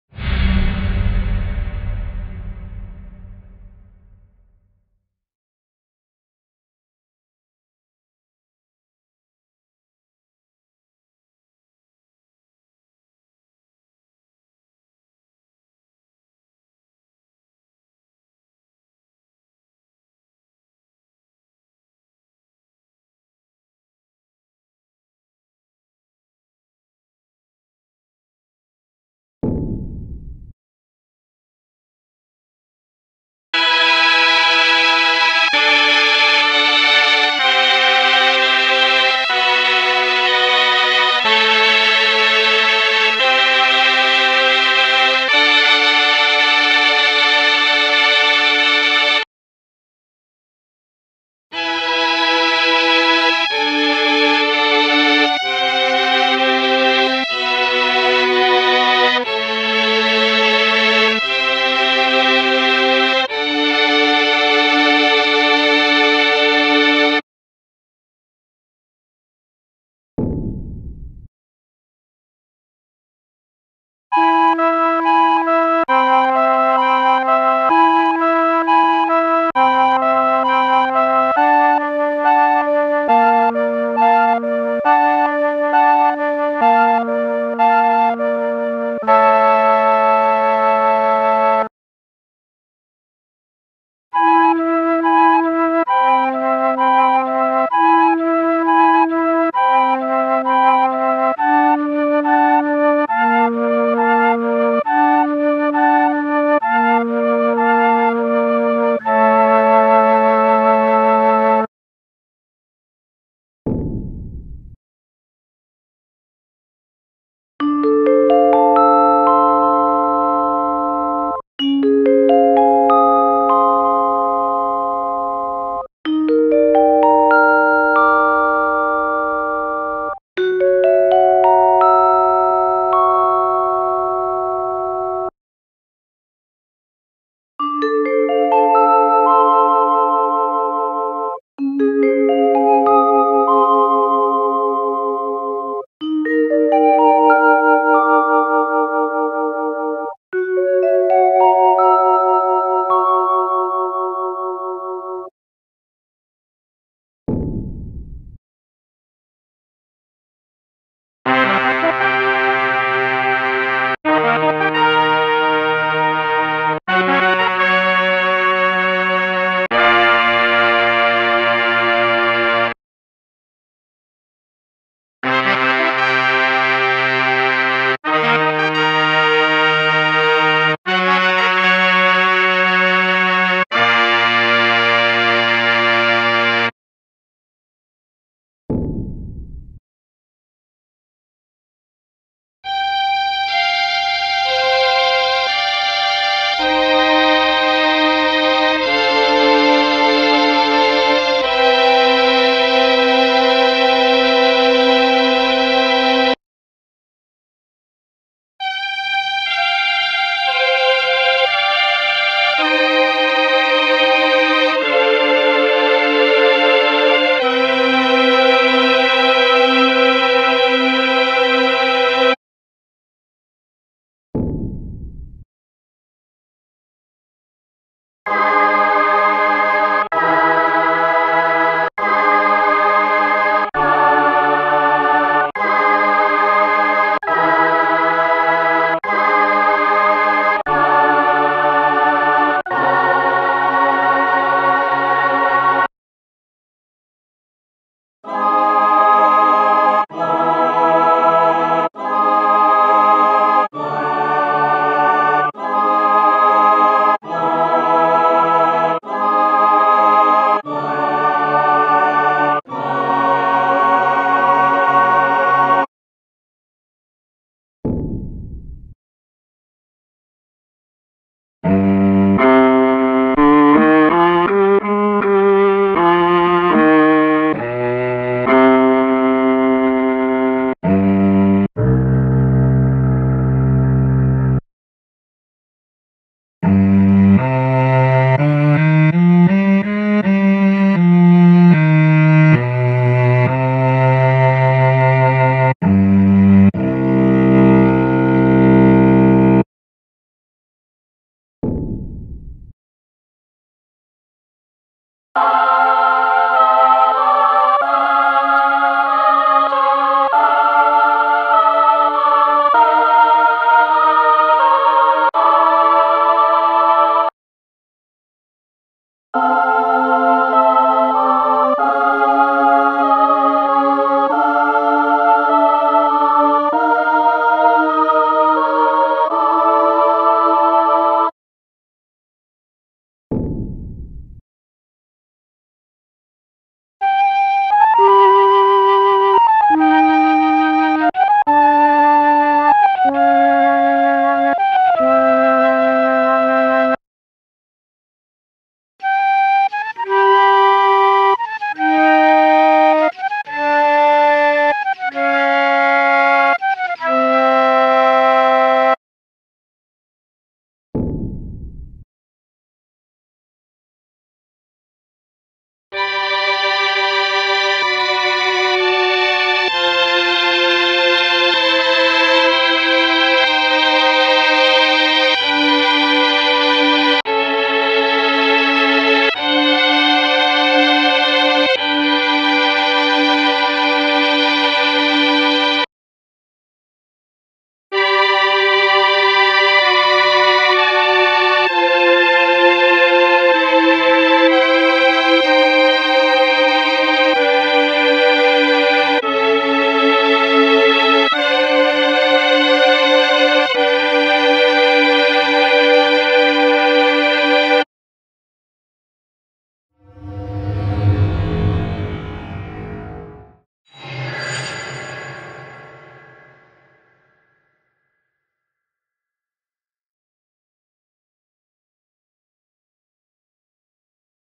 SampleTron 2 VS MELLOTRON V (Sample Sound Comparison)
Compare the sample sounds of IK Multimedia "SampleTron 2" and Arturia "MELLOTRON V".
Initializes the preset sound and turns off the effect.
Compare several similar sounds based on the sample name.

There is a difference in the types of sample sounds recorded by both.
"SampleTron 2" has more samples.
Even sample sounds with the same name have different tones.

IK Multimedia "SampleTron 2" とArturia "MELLOTRON V"のサンプル音の比較をしています。
プリセット音を初期化し、エフェクトをオフにした上で、サンプル名をもとに数種類の似ている音を比較しました。

両者の収録しているサンプル音の種類は違いが有り、"SampleTron 2"の方がサンプル数が多いです。
同じ名前のサンプル音でも音色が違っているので、似ている音色を選択し、比較しています。